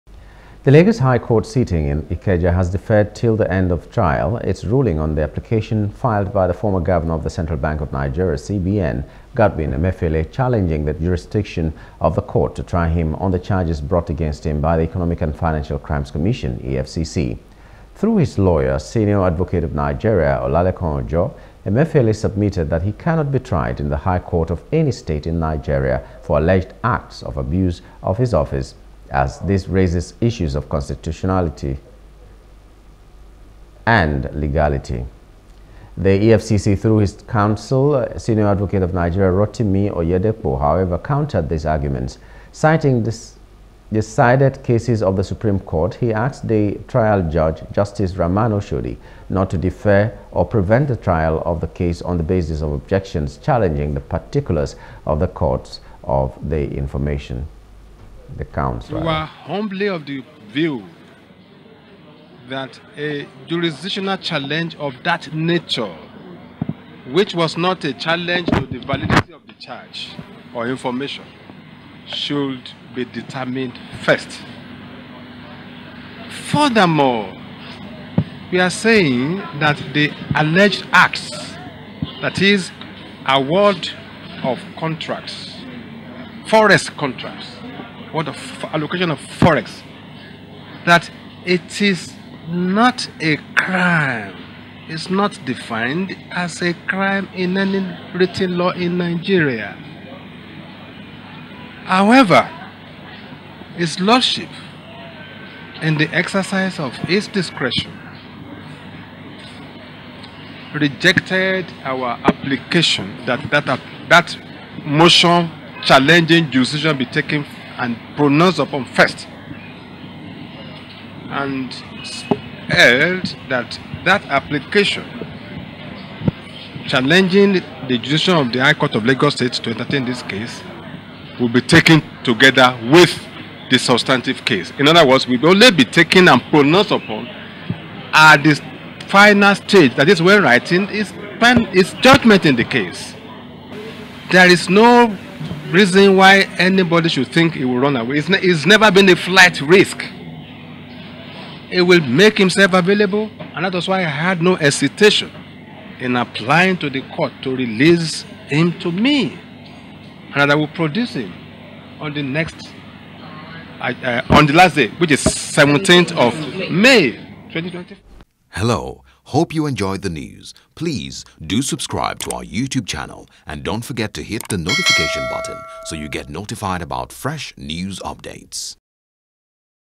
Alleged Abuse Of Office Emefiele Challenges Jurisdiction Of Lagos Court To Try Him
The Lagos High Court sitting in Ikeja has deferred till the end of trial, its ruling on the application filed by the former Governor of the Central Bank of Nigeria (CBN), Godwin Emefiele, challenging the jurisdiction of the court to try him on the charges brought against him by the Economic and Financial Crimes Commission (EFCC).